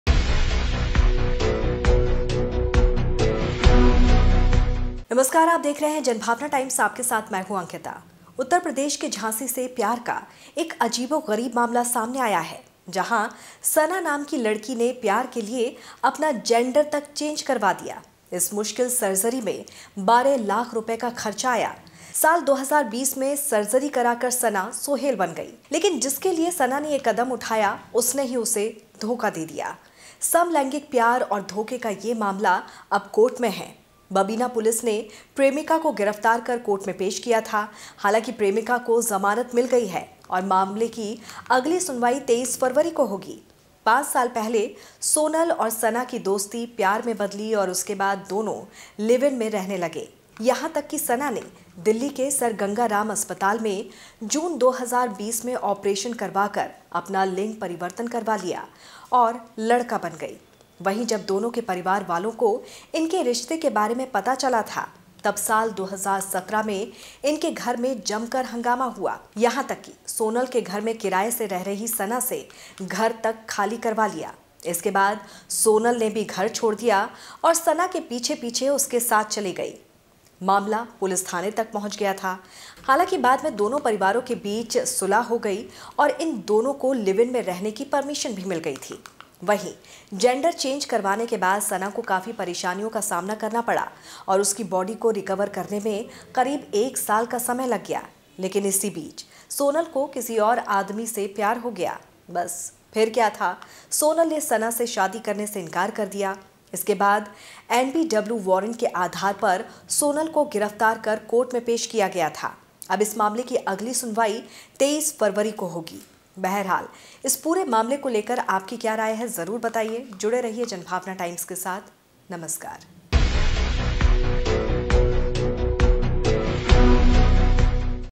Lesbian Couple | 5 साल के Relationship के बाद Couple पहुंचा Court | JBT News
उत्तर प्रदेश के झांसी से प्यार का एक अजीबोगरीब मामला सामने आया है जहां सना नाम की लड़की ने प्यार के लिए अपना जेंडर की चेंज करवा दिया इस मुश्किल सर्जरी में 12 लाख रुपए का खर्च आया। साल 2020 में सना एजाज अली सहित सोहेल बन गई। लेकिन के लिए सना ने यह कदम उठाया वही उसे धोखा देकर चली गई।

A strange case of love has come to light from Jhansi, Uttar Pradesh, where a girl named Sana got her gender changed for love. This difficult surgery cost Rs 12 lakh. In the year 2020, Sana became Sohail along with Eijaz Ali. But for Sana took this step, she betrayed him and went away.